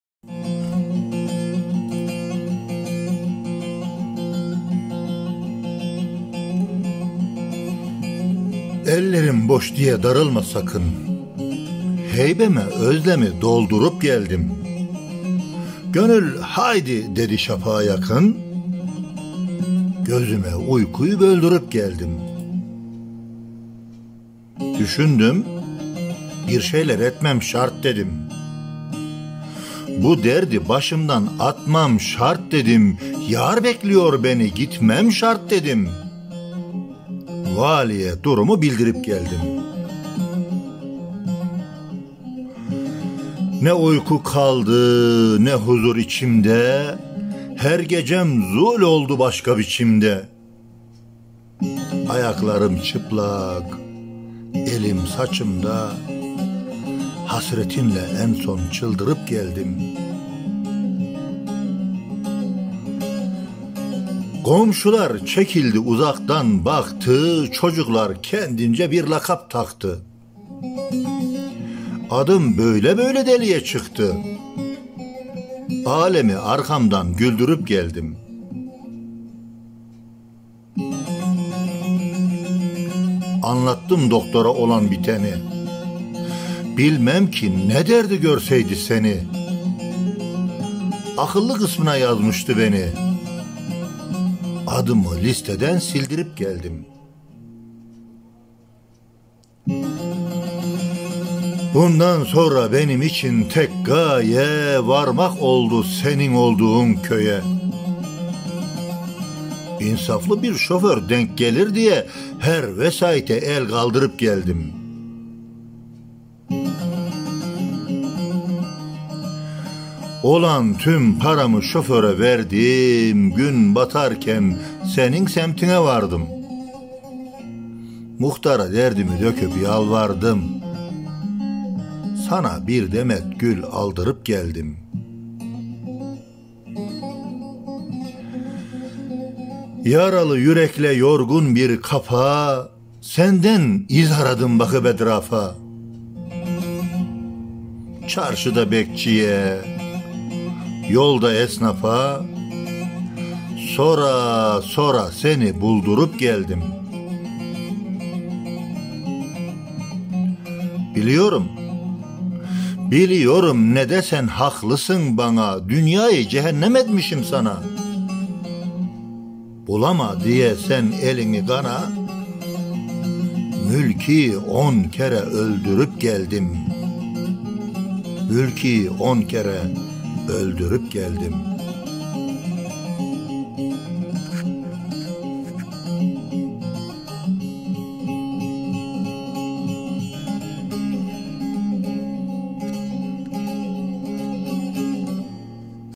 Ben geldim
Ellerim boş diye darılma sakın
Heybeme özlemi doldurup geldim
Gönül haydi dedi şafağa yakın
Gözüme uykuyu böldürüp geldim

Düşündüm bir şeyler etmem şart dedim
Bu derdi başımdan atmam şart dedim
Yâr bekliyor beni gitmem şart dedim
Valiye durumu bildirip geldim

Ne uyku kaldı ne huzur içimde
Her gecem zül oldu başka biçimde
Ayaklarım çıplak elim saçımda
Hasretinle en son çıldırıp geldim

Komşular çekildi uzaktan baktı
Çocuklar kendince bir lakap taktı
Adım böyle böyle deliye çıktı
Âlemi arkamdan güldürüp geldim

Anlattım doktora olan biteni
Bilmem ki ne derdi görseydi seni
Akıllı kısmına yazmıştı beni
Adımı listeden sildirip geldim

Bundan sonra benim için tek gaye
Varmak oldu senin olduğun köye
İnsaflı bir şoför denk gelir diye
Her vesaite el kaldırıp geldim

Olan tüm paramı şoföre verdim
Gün batarken senin semtine vardım
Muhtara derdimi döküp yalvardım
Sana bir demet gül aldırıp geldim

Yaralı yürekle yorgun bir kafa
Senden iz aradım bakıp etrafa
Çarşıda bekçiye yolda esnafa
Sora sora seni buldurup geldim

Biliyorum ne desen haklısın bana
Dünyayı cehennem etmişim sana
Bulama sen diye elini kana
Mülkî’yi on kere öldürüp geldim

Aslan AVŞARBEY